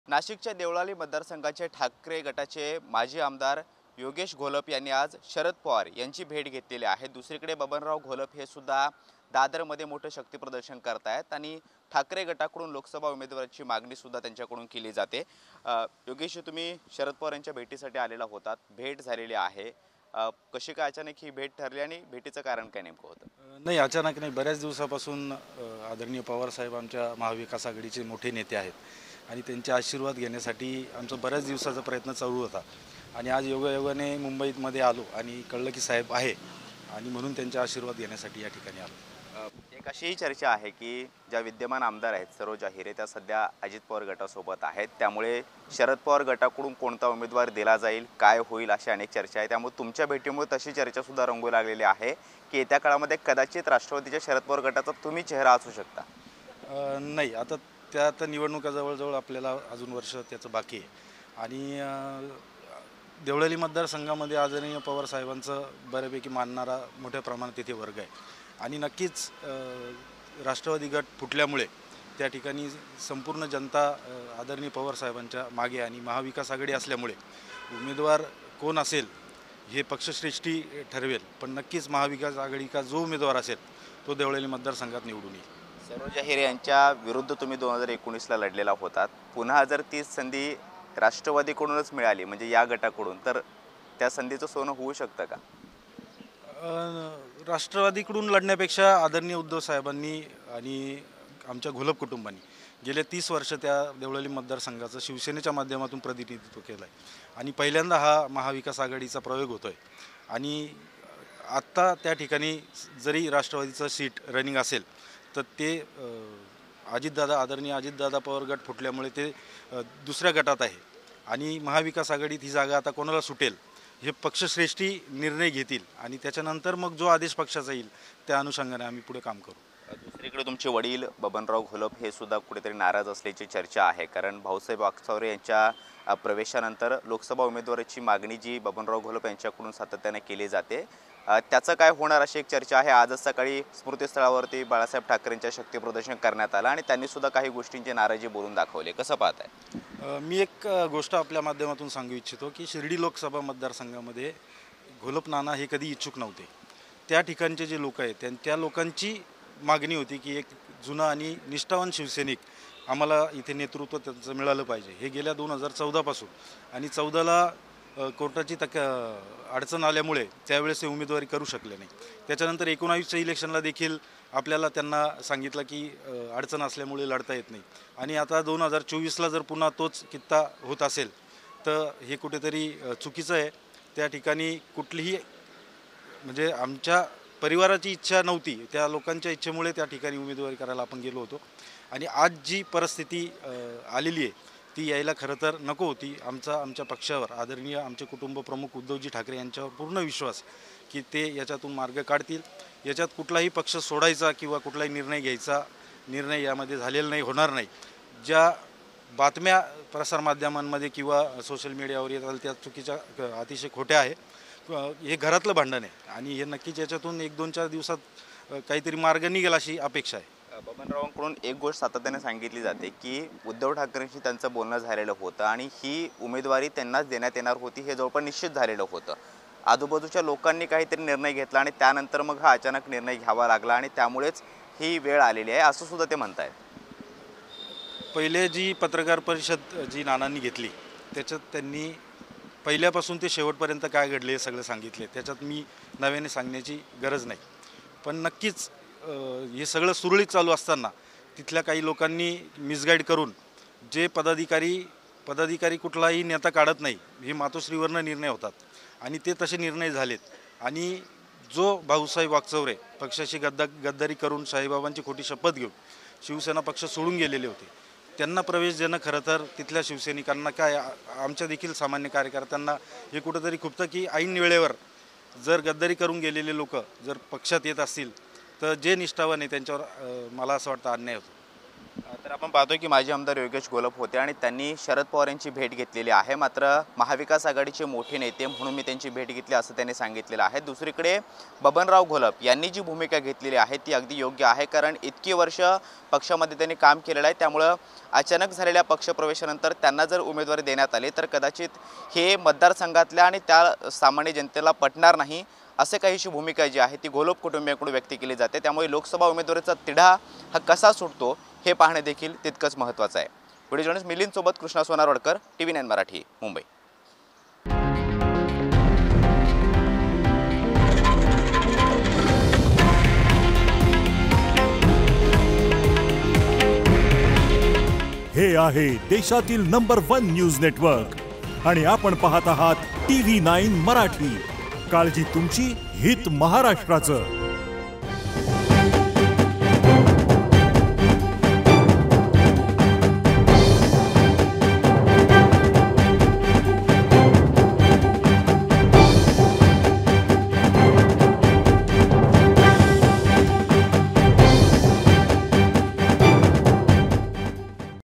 माजी आमदार Yogesh Gholap Sharad Pawar यांच्या भेटीला, दोघांमध्ये काय चर्चा झाली?
CREDIT - Political| Preeti Mardhekar /Producer|Joseph/Editor